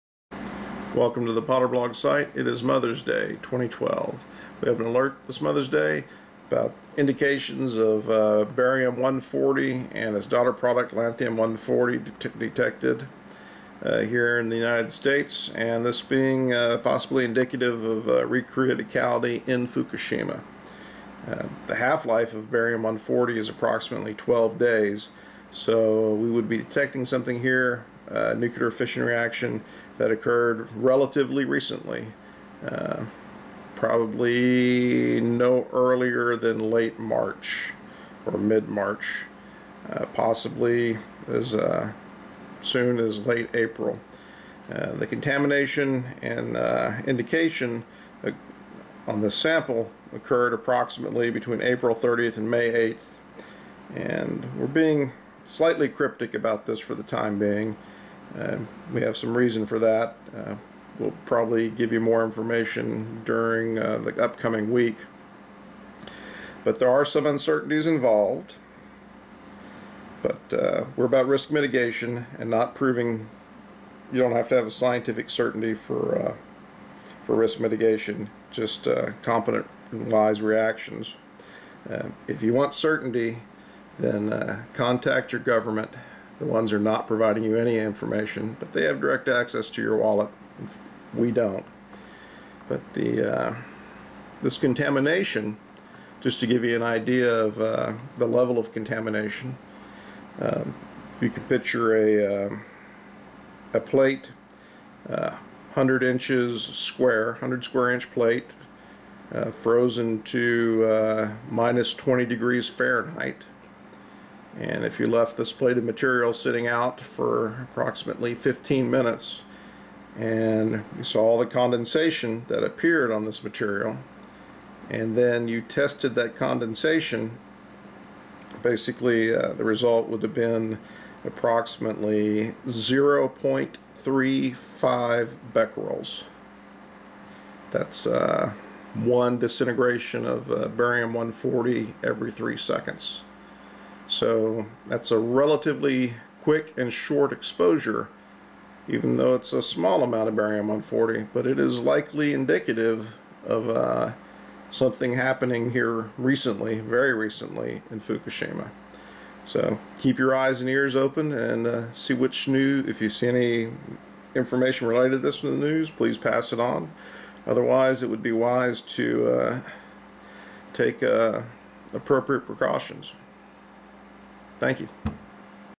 ALERT Recent Significant Fukushima Recriticality: Indications Of Barium-140
There are indications of Barium-140 and La-140 contamination and detection occurring in the USA between April 30th and May 8th of 2012.  The approximate 12 day half life of Ba-140 points to a  SIGNIFICANT and RECENT nuclear reaction, likely a recriticality from Fukushima.